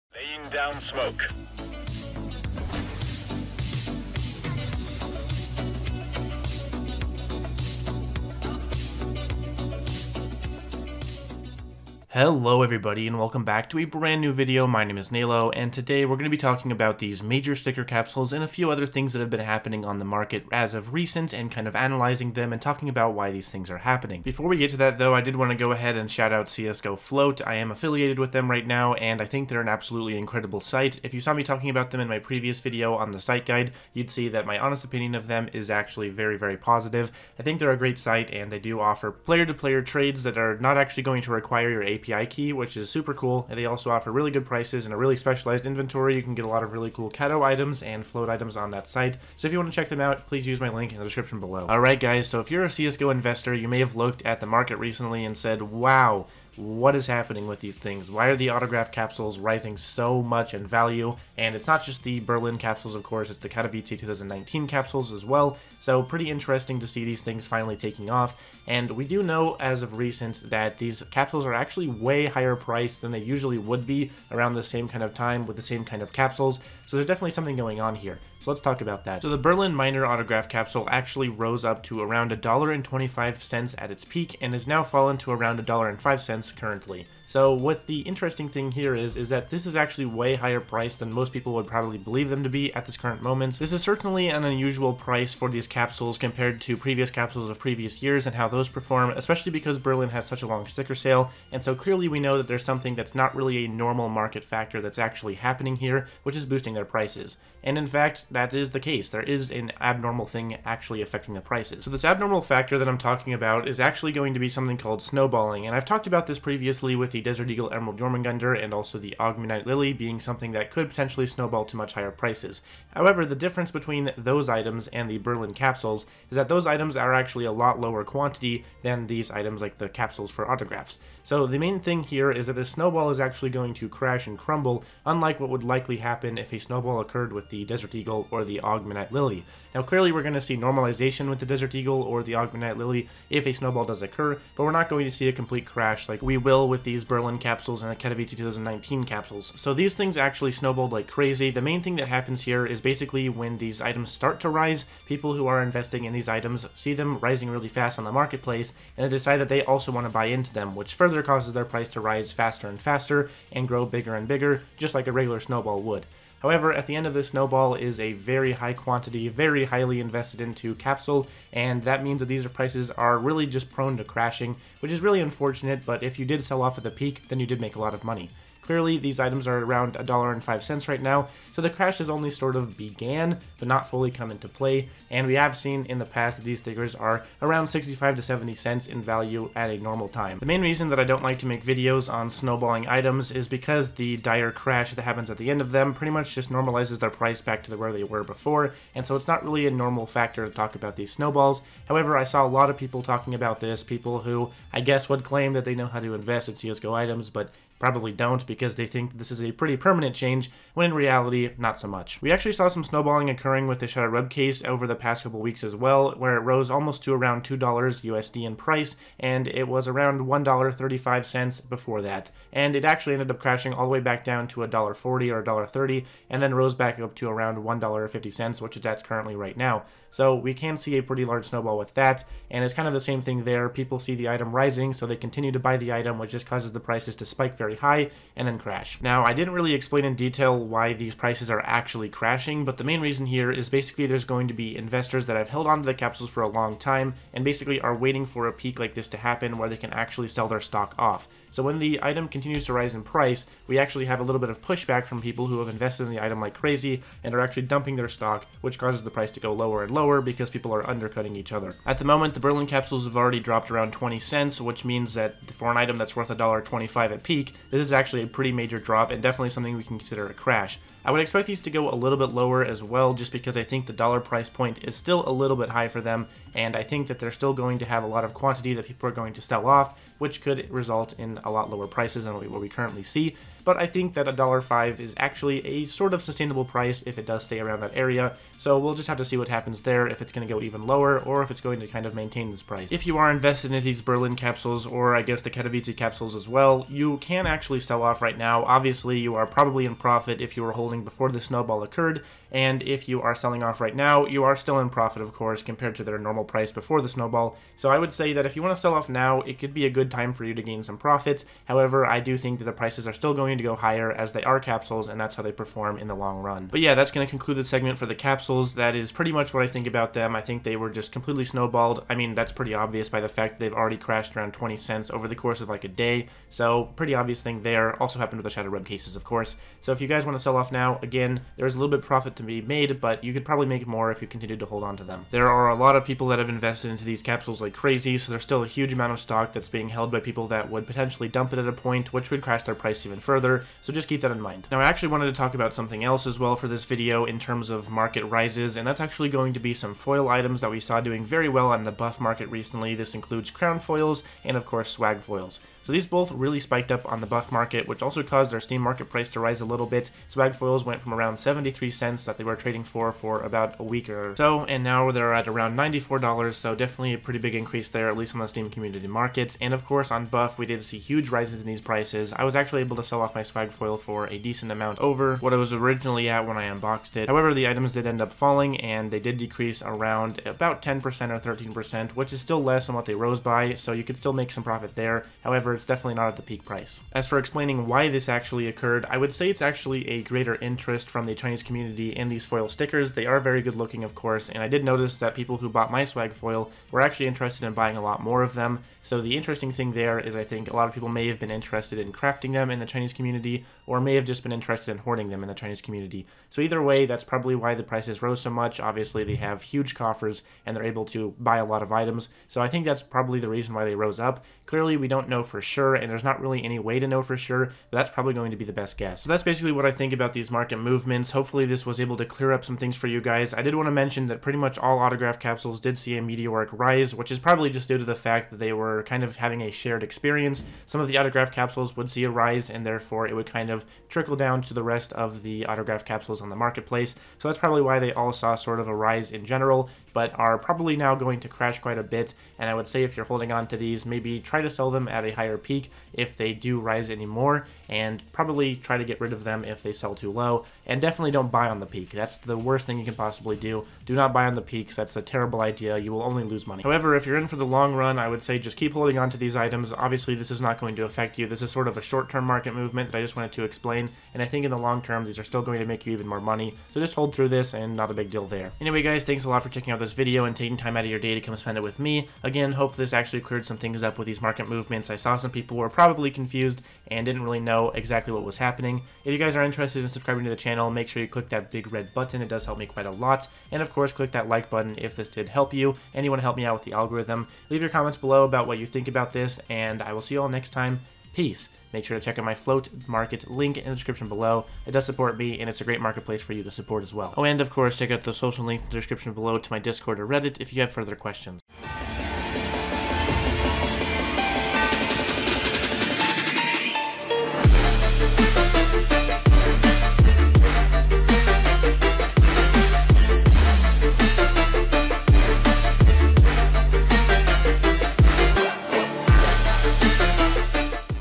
MARKET MOVEMENTS ft. AUTOGRAPH CAPSULES & FOILS | The Snowball
We are seeing some crazy market movements with these autograph capsules and some foils on buff! Lets talk about these movements and analyze what the future looks like for these items!

//SOCIALS:
SOCIALS\\

//NALO
NALO\\